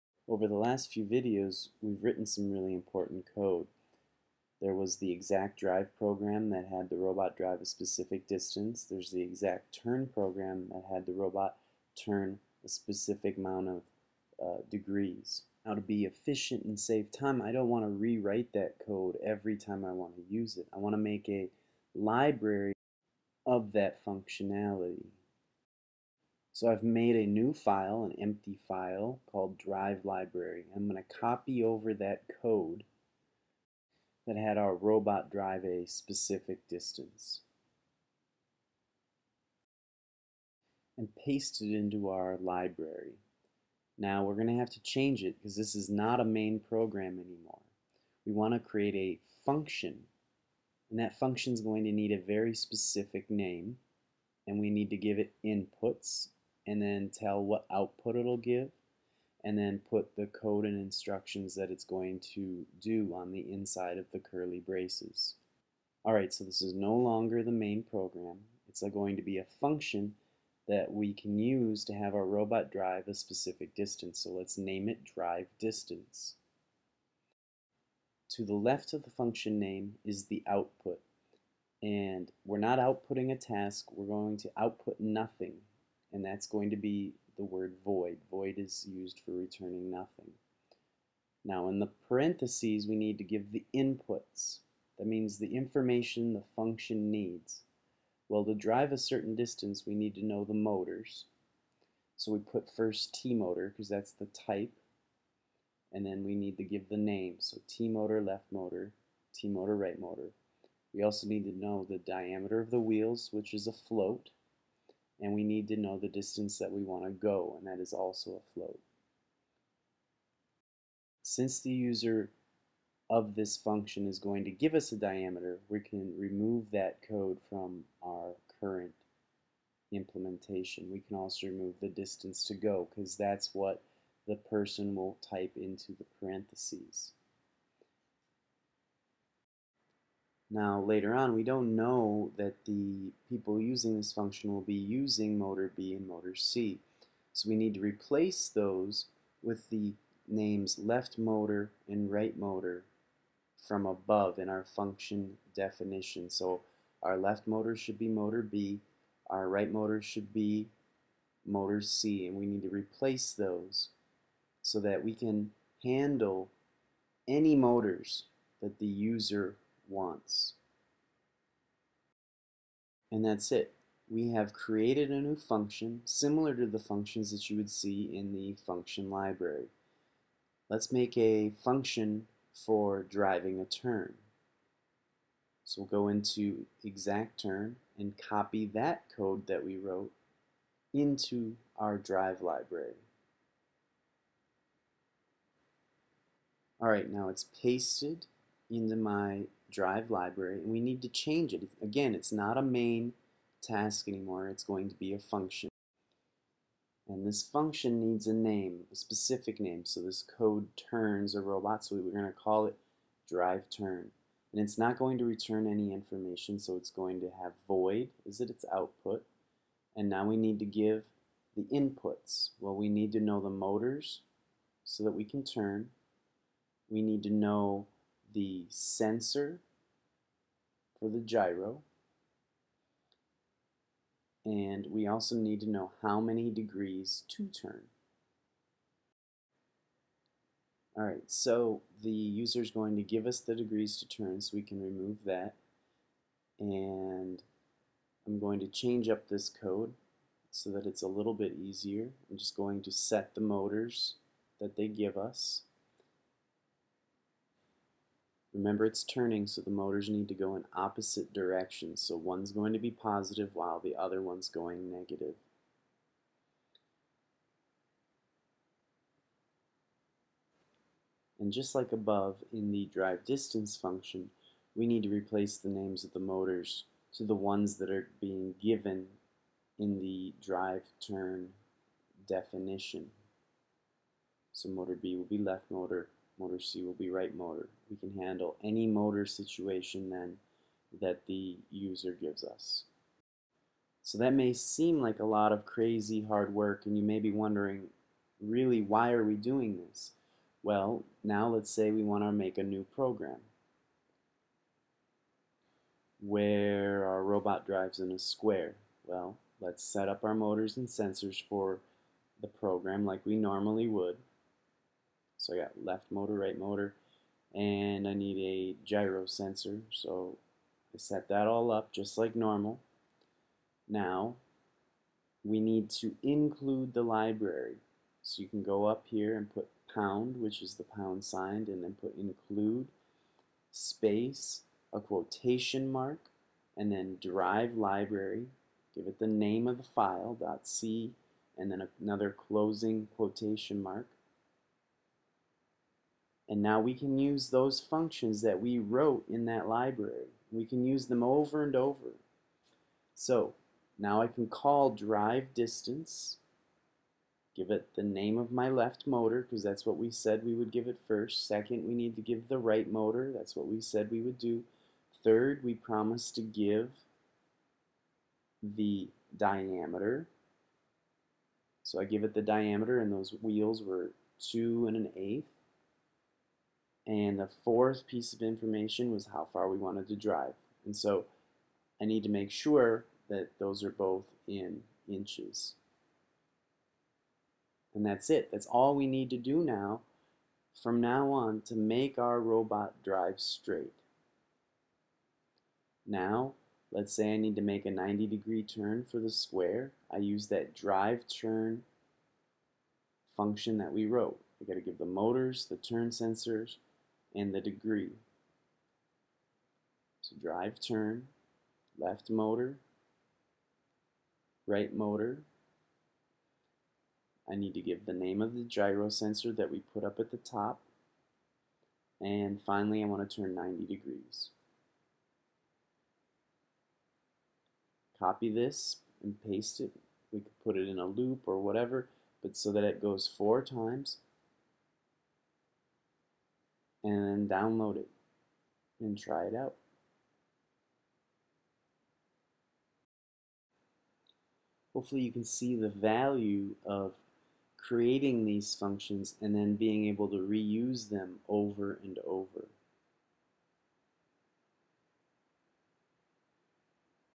Intro to RobotC 8: Functions
Shows the basic idea behind writing functions in RobotC.